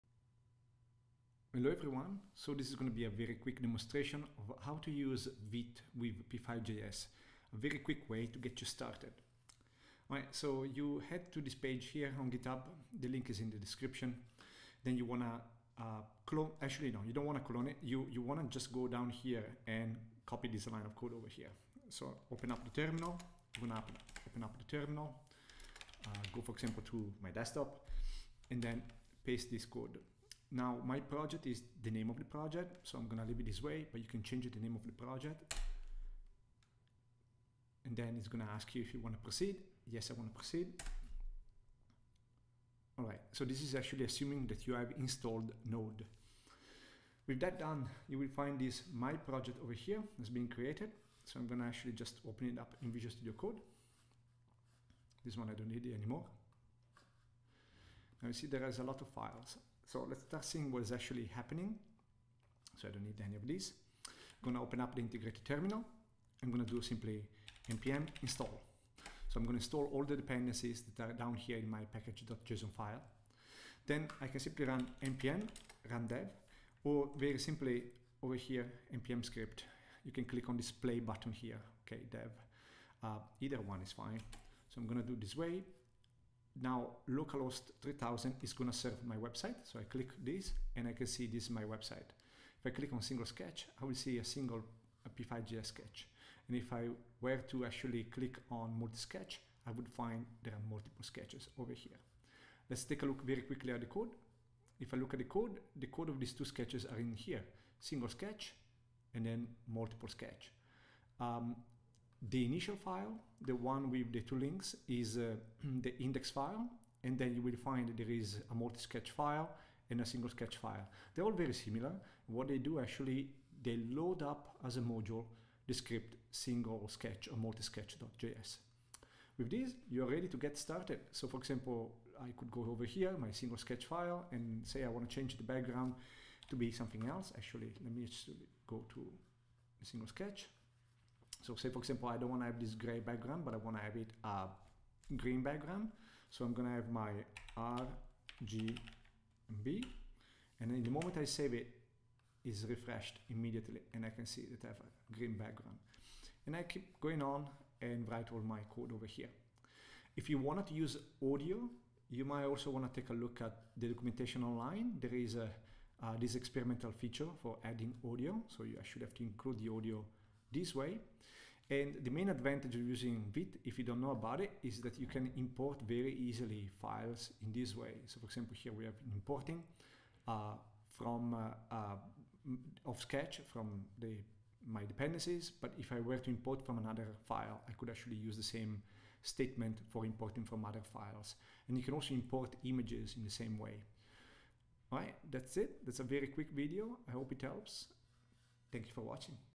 Getting started with Vite + p5.js
Quick tutorial showing how to create a sketch with P5.js using Vite.